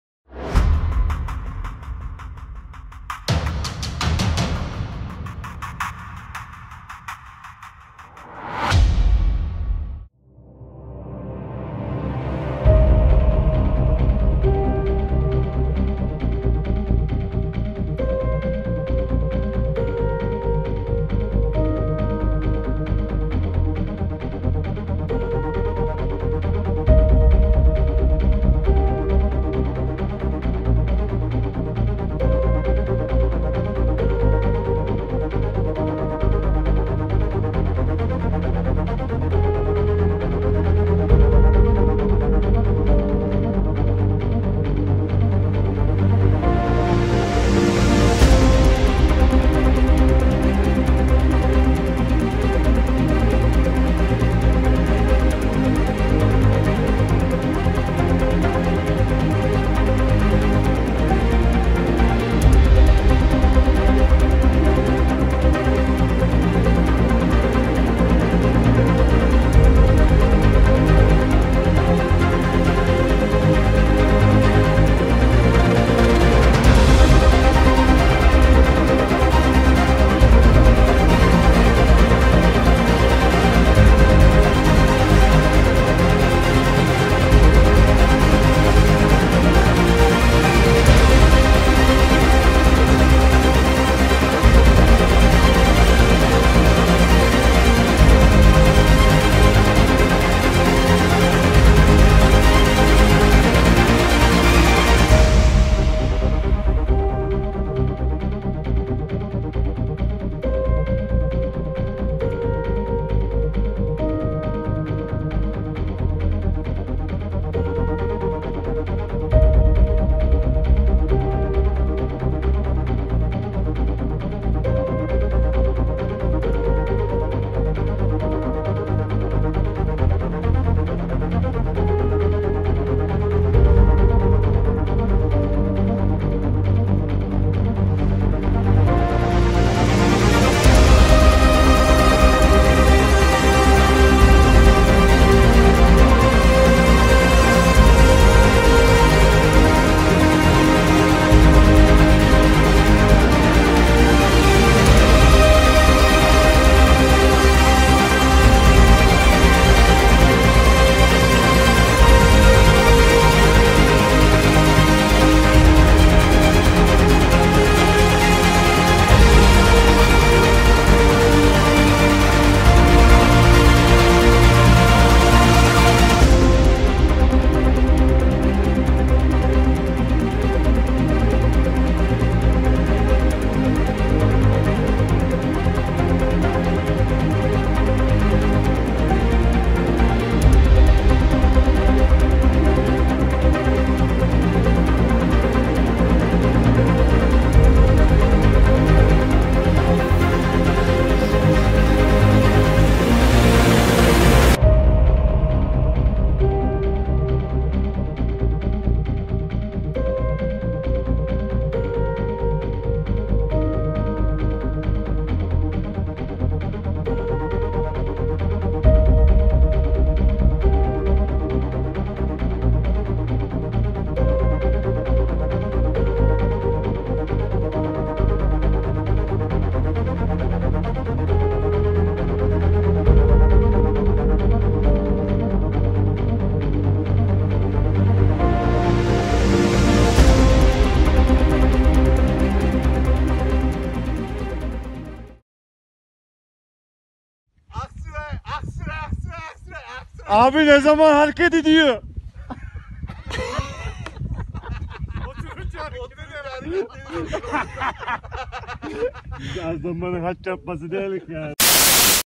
Erciyes: Sidecountry Paradise
A group of friends snowboarding/skiing classical lines in bounds and sidecountry of the Hitittepe, Lifos, Divan and Devil's Creek, Mount Erciyes. Thanks to every fella for another awesome week of snowboarding/skiing. Freedom for all :)